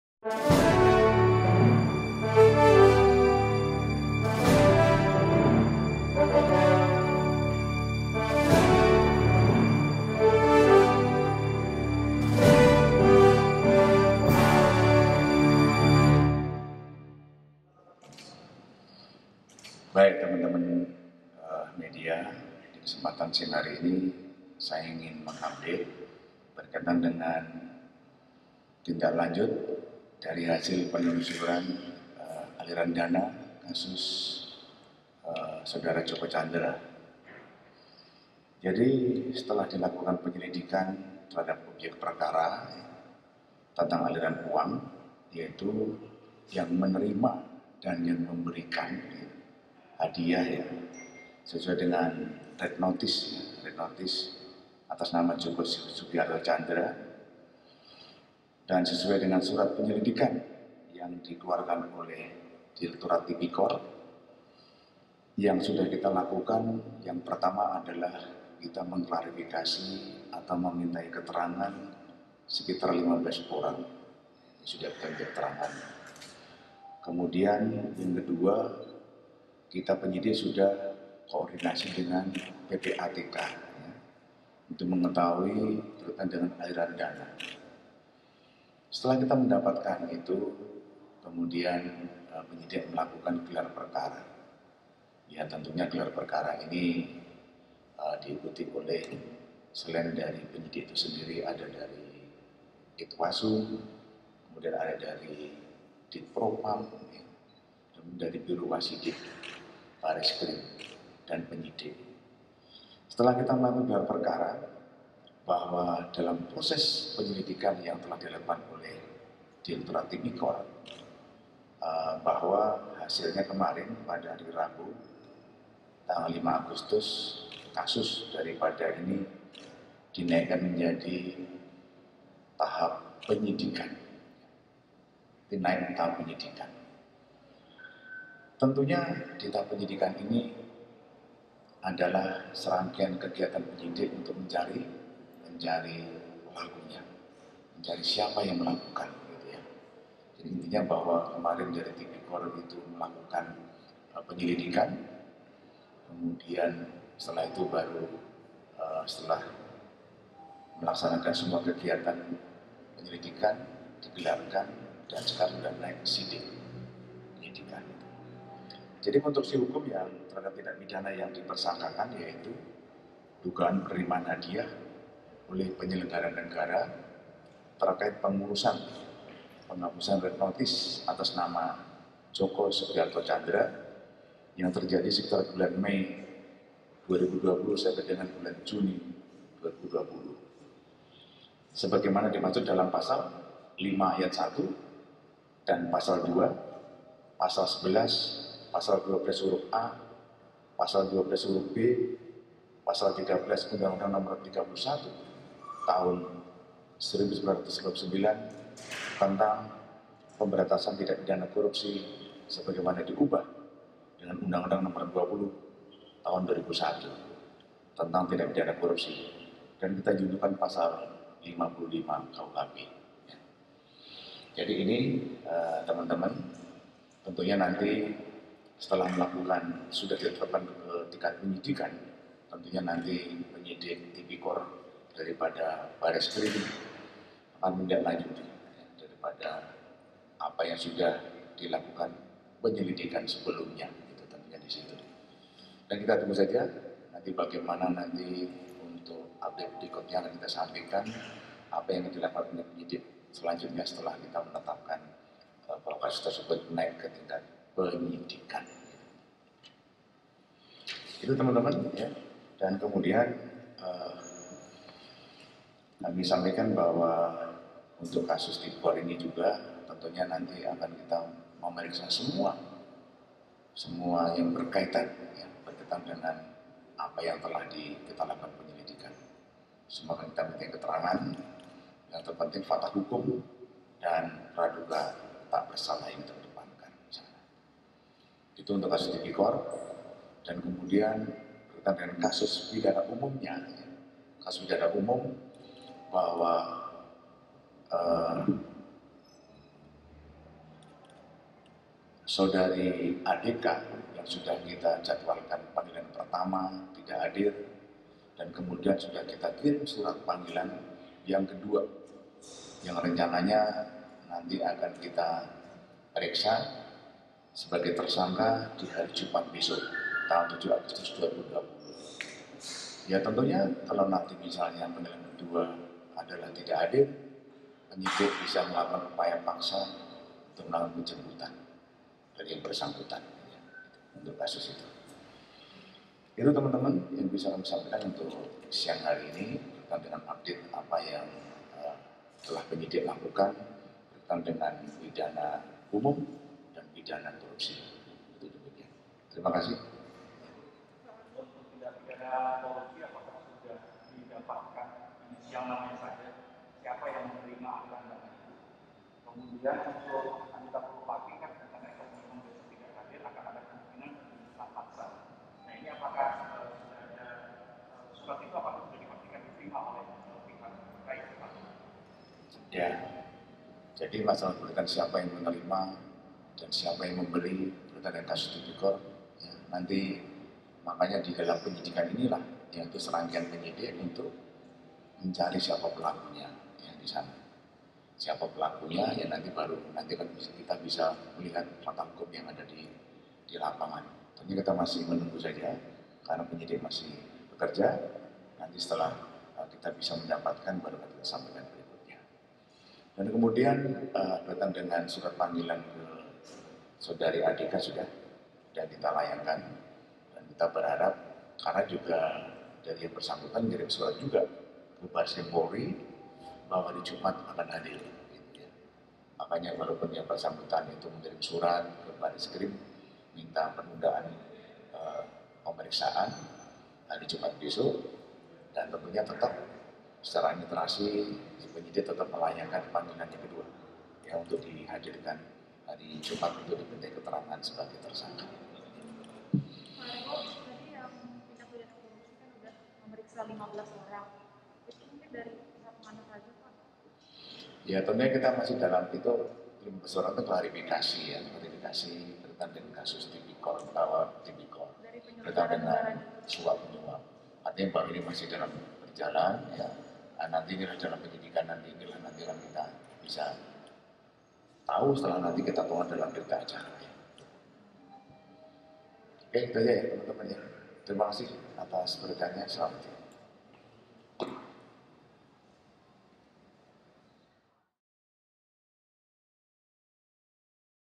Konferensi Pers Tindak Lanjut Penelusuran Aliran Dana Tjoko Chandra
Konferensi Pers

Kadiv Humas Polri, Irjen. Pol. Raden Prabowo Argo Yuwono, S.I.K., (06/08/2020).

Terkait Tindak Lanjut Penelusuran Aliran Dana Tjoko Chandra.

Setelah dilakukan penyelidikan perkara, yang menerima dan memberikan hadiah sesuai _*Red Notice*_ atas nama Djoko Soegiarto Tjandra dan sesuai surat penyelidikan.